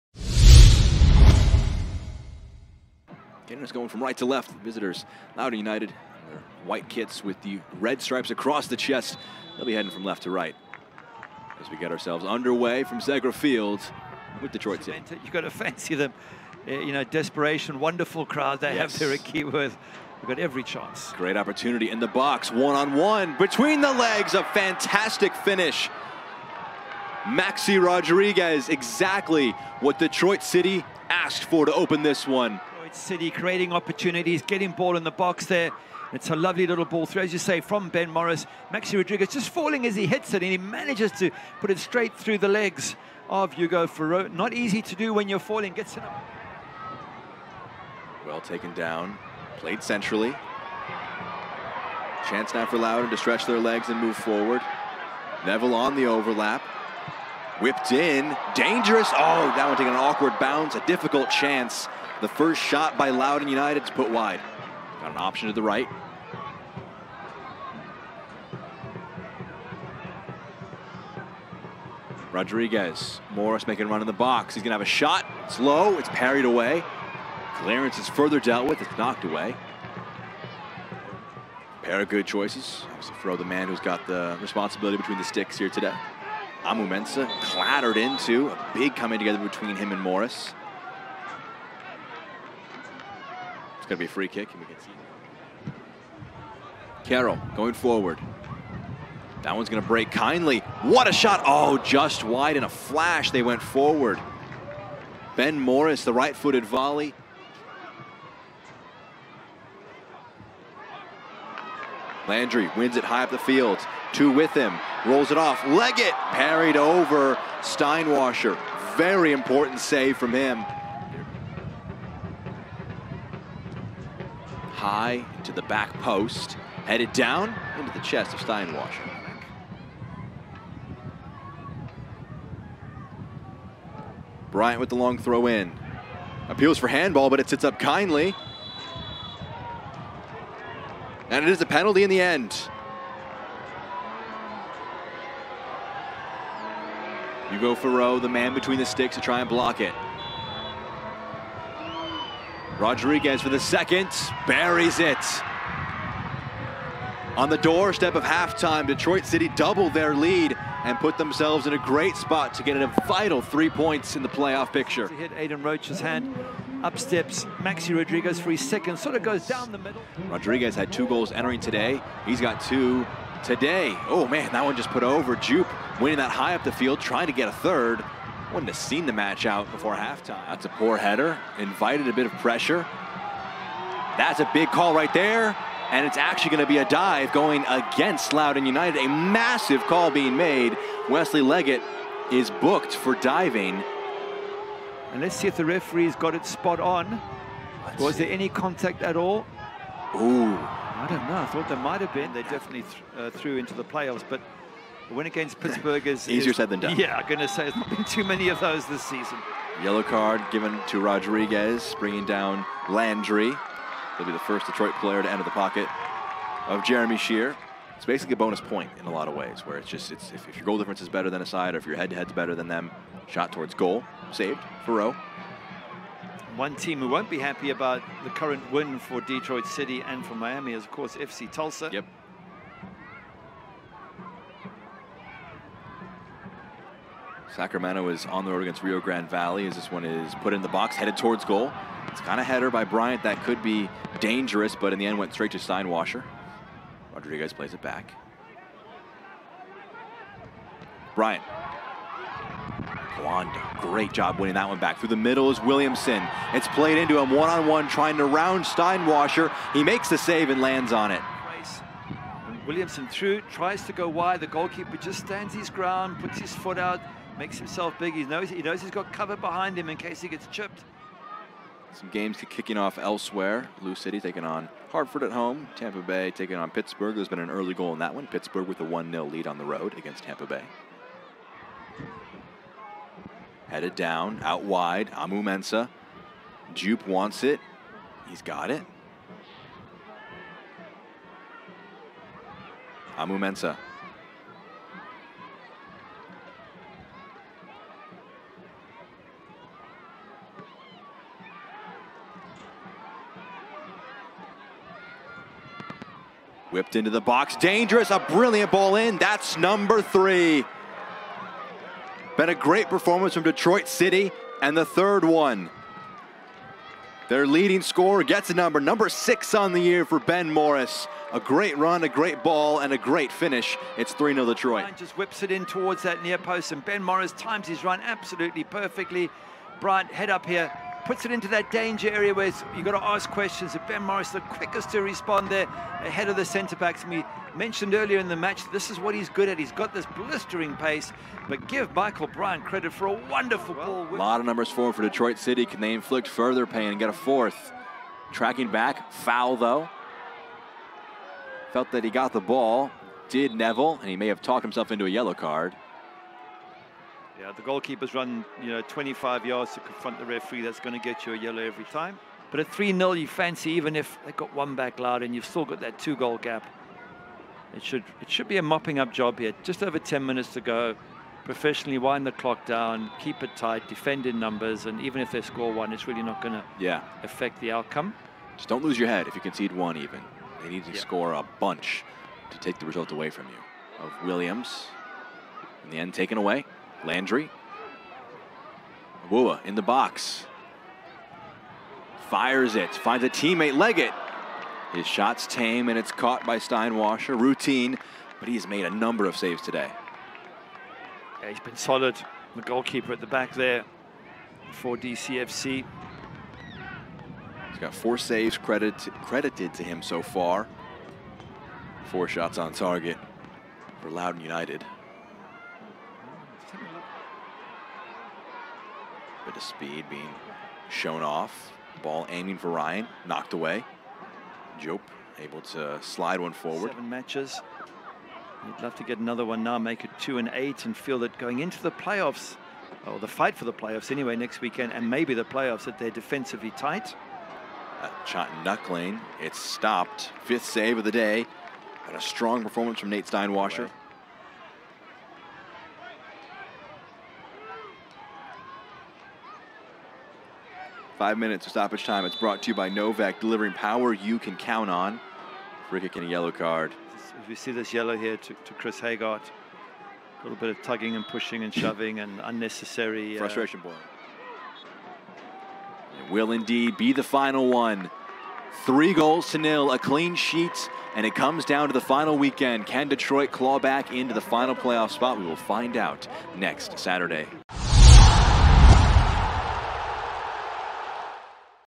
10.7.2023 | Loudoun United FC vs. Detroit City FC - Game Highlights
Maxi Rodriguez scored a pair of goals as Detroit City FC took a 3-0 victory against Loudoun United FC at Segra Field to remain in the hunt for the final postseason berth in the Eastern Conference.

00:00:00 - Kick Off by Detroit City FC
00:00:15 - Goal by Maxi Rodriguez
00:00:46 - Pass by Abdoul Koanda
00:00:53 - Shot by Jake Morris
00:01:05 - Pass by Abdoulaye Diop
00:01:12 - Pass by Maxi Rodriguez
00:01:21 - DefensiveAct by Jake Morris
00:01:34 - Shot by Ben Morris
00:01:41 - Pass by Stephen Carroll
00:01:47 - GK Save by Nathan Steinwascher
00:02:01 - Shot by Wesley Leggett
00:02:10 - Foul by Aidan Rocha
00:02:23 - Goal by Maxi Rodriguez
00:02:49 - Shot by Abdoulaye Diop
00:02:56 - DefensiveAct by Abdoulaye Diop
00:03:00 - Booking by Wesley Leggett
00:03:22 - Booking by Maxi Rodriguez
00:03:38 - GK Save by Hugo Fauroux
00:03:52 - DefensiveAct by Yanis Leerman
00:04:07 - DefensiveAct by Abdoul Koanda
00:04:14 - Shot by Tommy Williamson
00:04:31 - DefensiveAct by Houssou Landry
00:04:41 - Pass by Devon Amoo-Mensah
00:04:50 - Cross by Abdoulaye Diop
00:05:00 - Goal by Ben Morris
00:05:44 - Booking by Hugo Fauroux
00:06:02 - Foul by Brett Levis
00:06:13 - Booking by Brett Levis
00:06:24 - Pass by Houssou Landry
00:06:30 - Shot by Wesley Leggett
00:06:40 - Pass by Nathan Steinwascher
00:06:57 - Cross by Isaac Espinal
00:07:07 - Pass by Cole Turner
00:07:15 - Shot by Christopher Hegardt
00:07:24 - Booking by Christopher Hegardt
00:07:44 - End Match by Detroit City FC